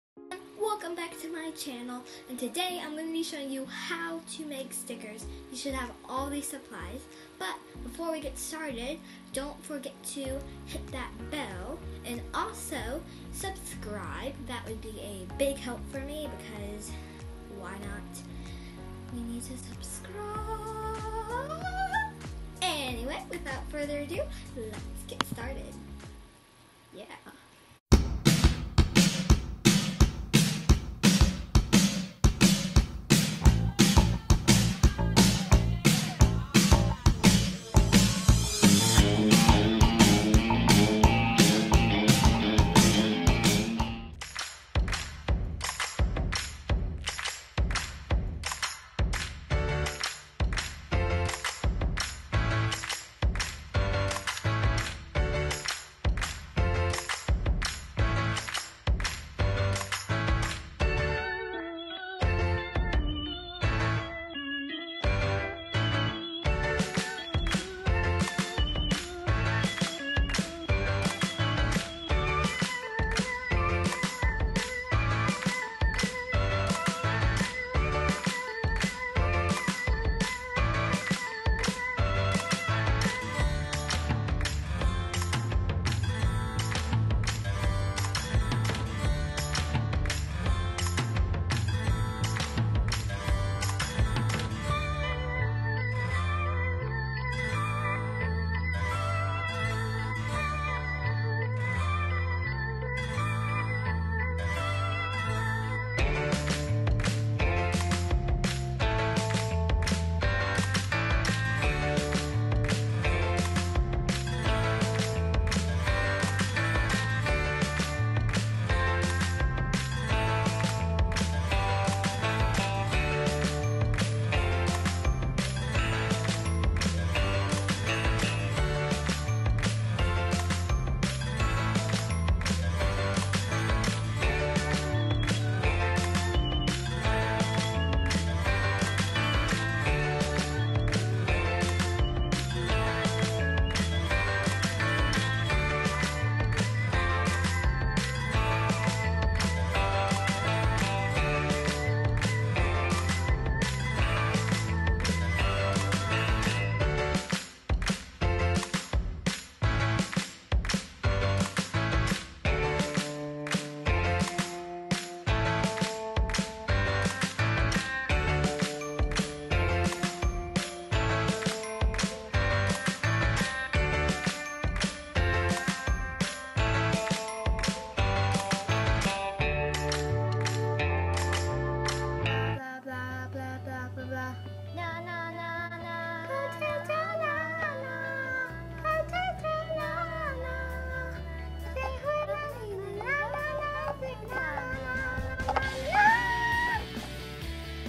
How to make stickers with supplies everyone has!
I really love this idea! It's also so satisfying to peel of the sticker to me, I hope you guys have a awesome day. Ta ta for now! ♡